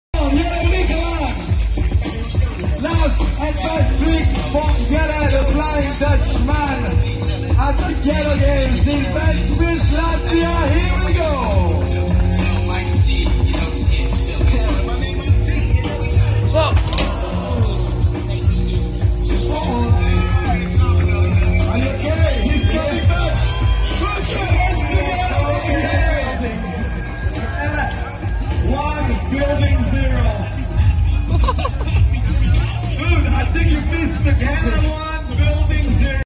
Rollerblade over the wall fail
Rollerblade over the wall fails at the local competition.